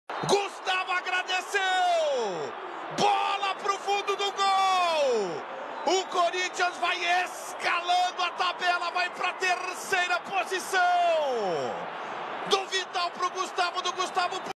Corinthians 1x0 Atlético MG | Gol de Gustavo | Brasileirão 2019
Gol Gustavo
Gol Corinthians